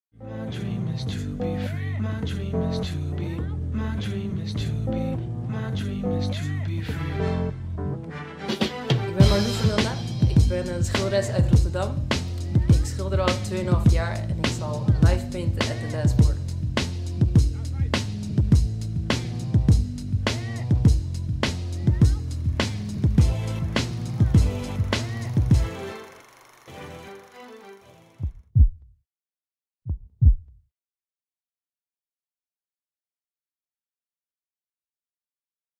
The Last Word Festival Marlou Drawing
Op 5 juni komt Marlou Drawing live painten op het festival.